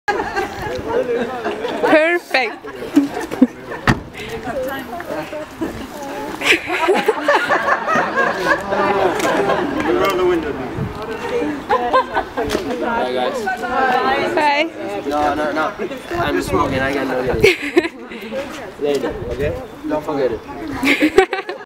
Jauja movie presentation with Viggo Mortensen and Ghita Nørby
And what happens behind the scenes...